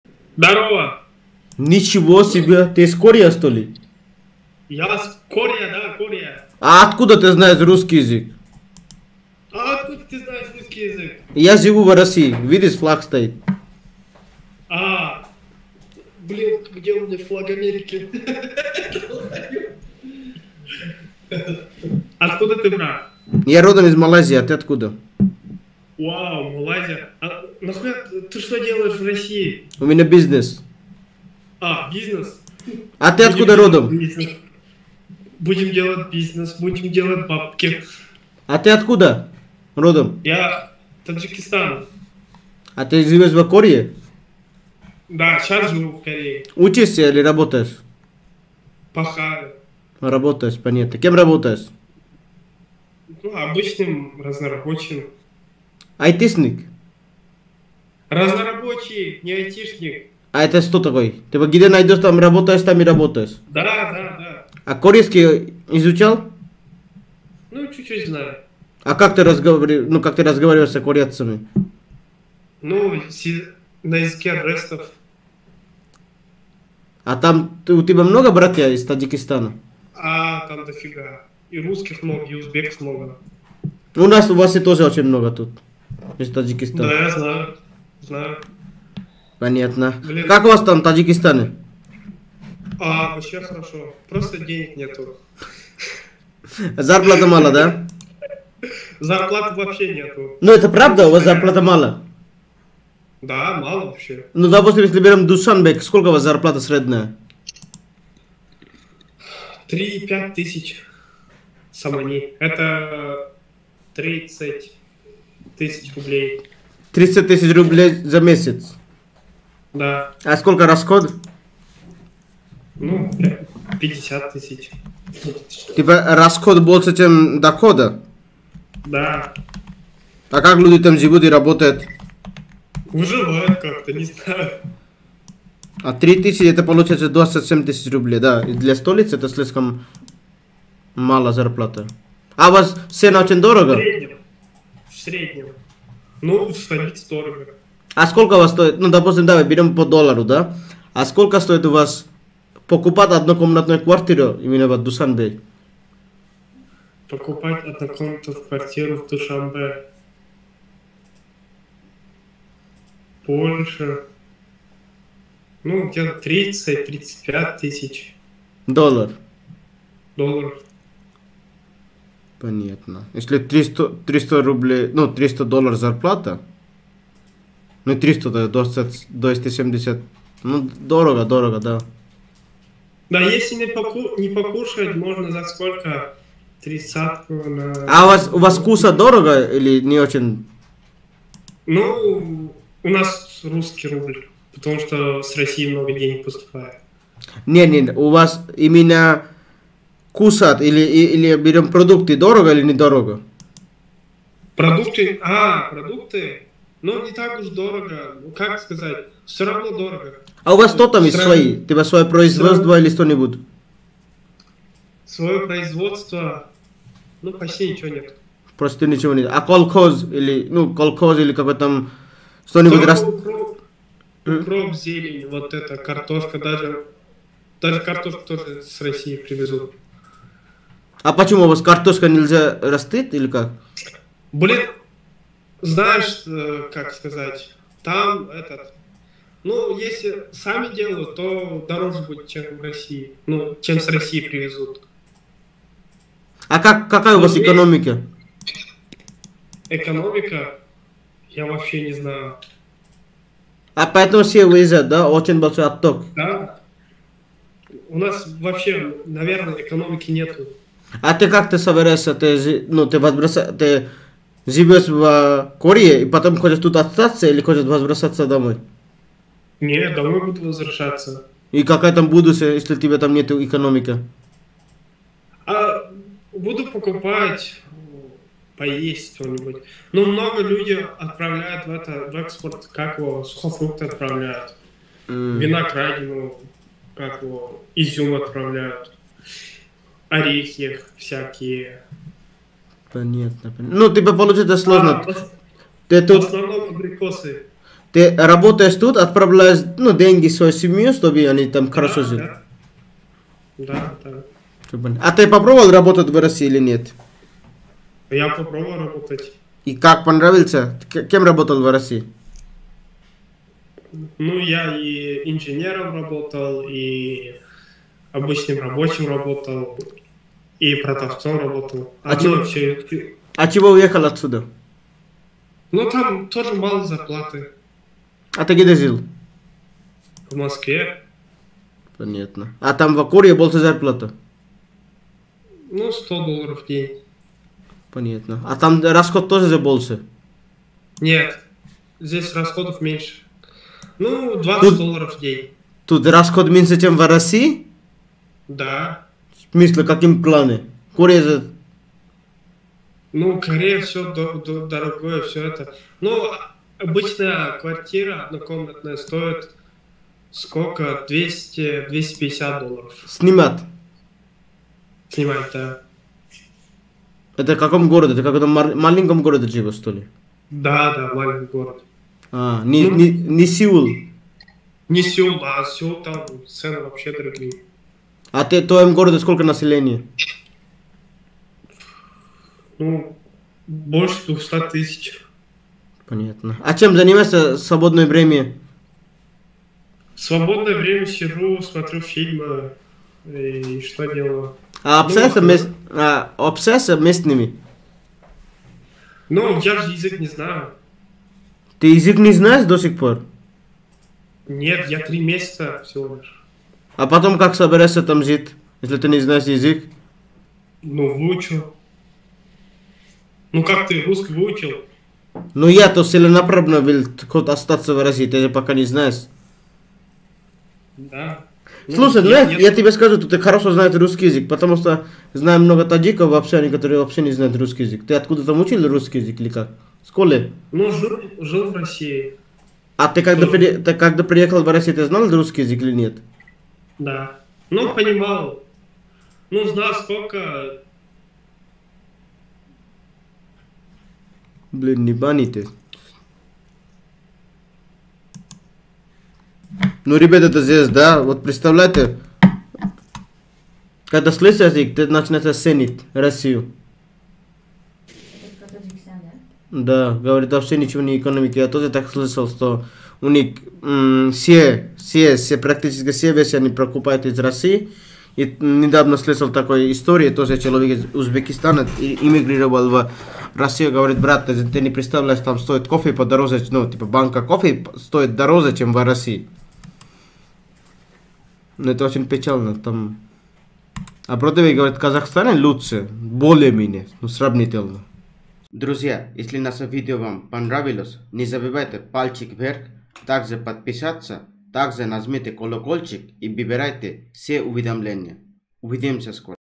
Экономическая ситуаця в Таджикистане сейчас
Что делает таджик в Корее? Почему он не остался в Таджикистане? В каком состоянии экономика Таджикистана сейчас? Сравниваем с Россией в этом видео.

USDT (BSC BEP20): 0xc783c556b15098c022e50edb5a9d1101ef12d4f5
USDT (TRC 20): TXxZr5x1aWHVk2FcjZtdZL9Knf8F12hw4E
Ethereum ( ERC20): 0xc783c556b15098c022e50edb5a9d1101ef12d4f5